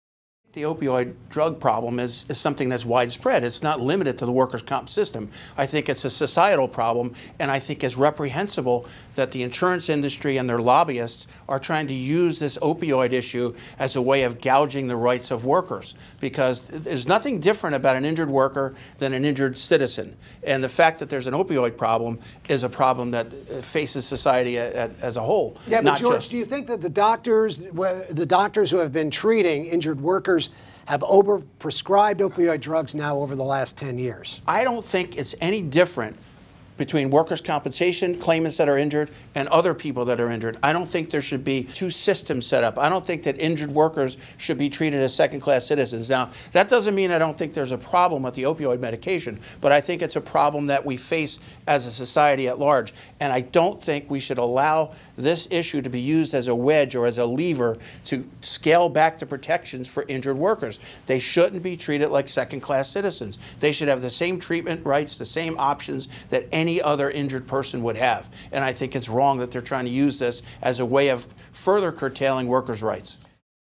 Opioid Epidemic & Workers' Comp: Atty. George Badey comments
What impact is the opioid epidemic having on workers' compensation? Atty. George Badey comments.